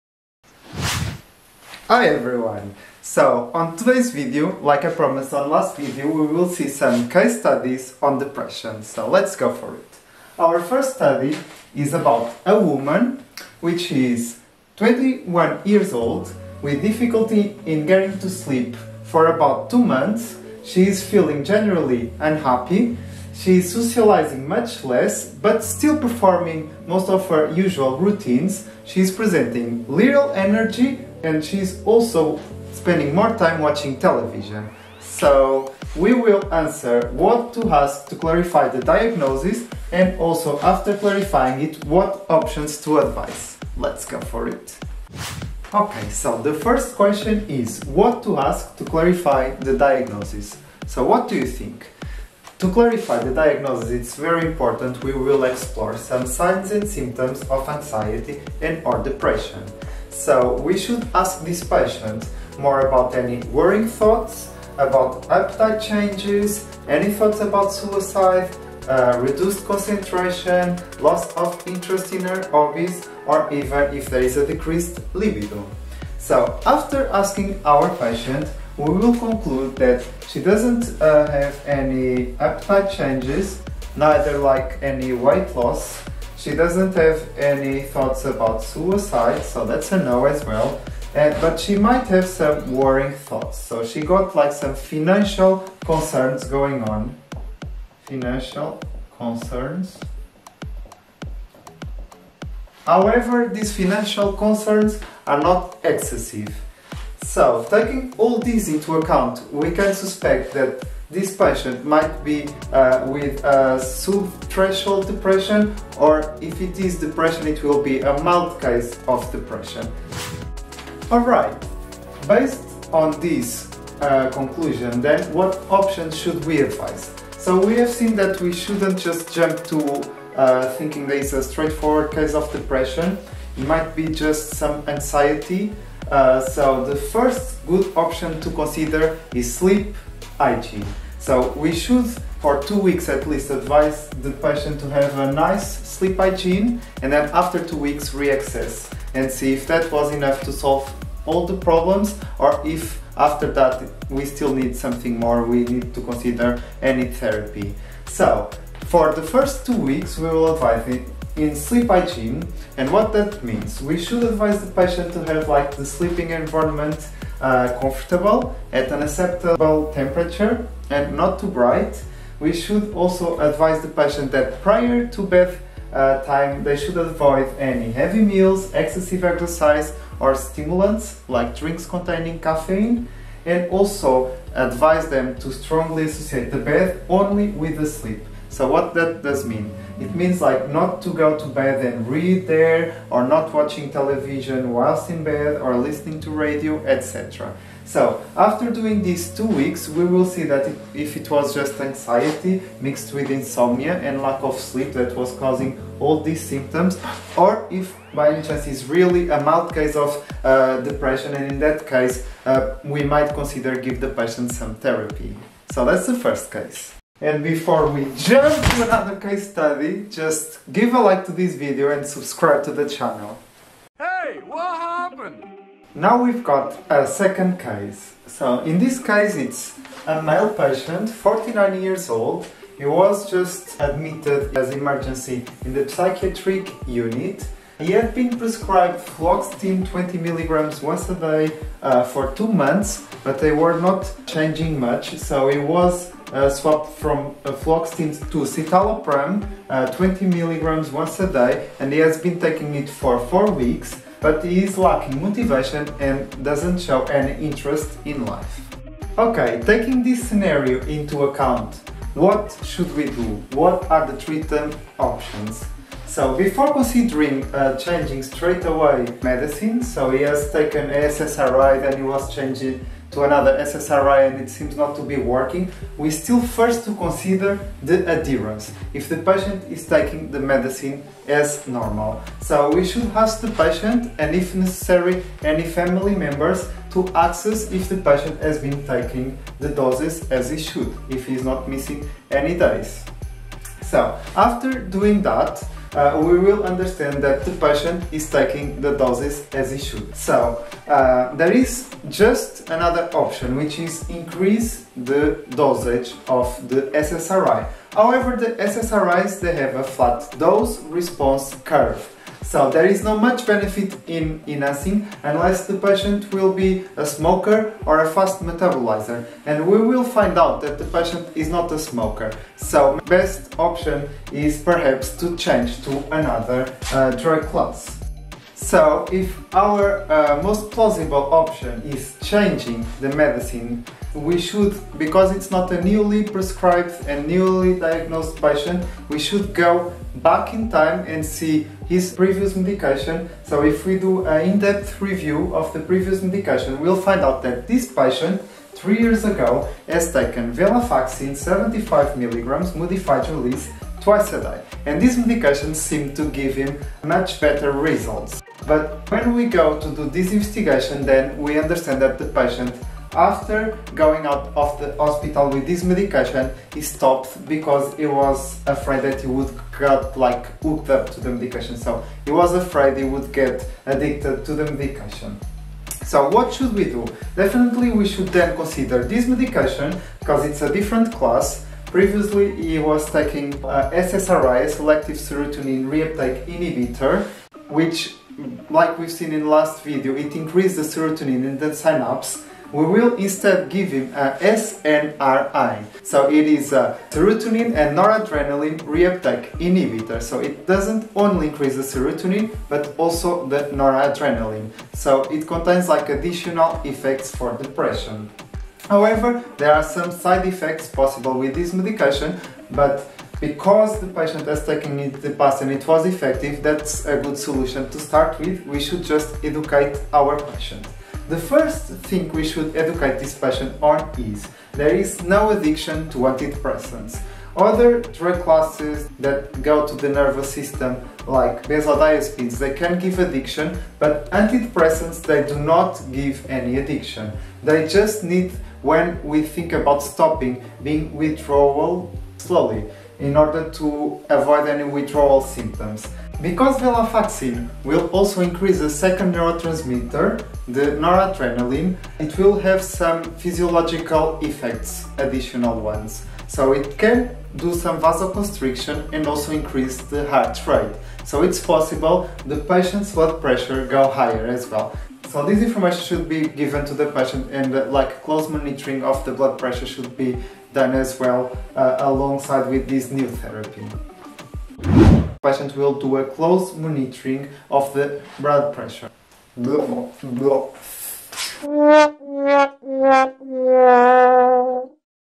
Antidepressants explained in Case Studies + Non-Pharmacological Tips
LET’S THINK TOGETHER.

After presenting all the antidepressant classes, it’s time to use the knowledge and think clinically (and critically) with the help of cases that we can be presented in practice.

On this first CASE STUDIES video, we bring you 2 different scenarios to discuss some concepts on depression and antidepressants when dealing with concrete cases in the ‘real’ world.
Main message is that each case in practice is different and sometimes situations may not be straightforward. Doctors, Pharmacists, Non-Medical Prescribing (either Pharmacists, Nurses or other Healthcare Professionals) should always make their best effort to approach each situation in a personalised way. The patient’s reality should be carefully considered, thus these examples intend to foster a critical thinking in parallel with a clinical judgment, based on solid information we might already know.
Basically combining what we know already (clinically) with what we did not know (and it is the particular case of a unique patient), from which it should result a patient-centred approach and critical decisions.

These case studies were adapted from Clinical Pharmacy and Therapeutics, 6th Edition.

Now we have a IG page too, so you can follow us there:

Easy Peasy Pharmacology